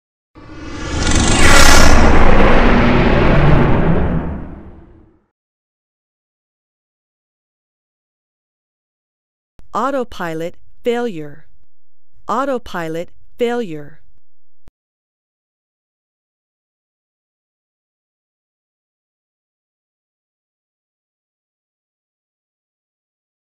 Autopilot Failure
Genesys Aerosystems Electronic Flight Instrument System (EFIS)
IDU-680 8.0F Fixed Wing and/or Rotorcraft
Audio: Autopilot Failure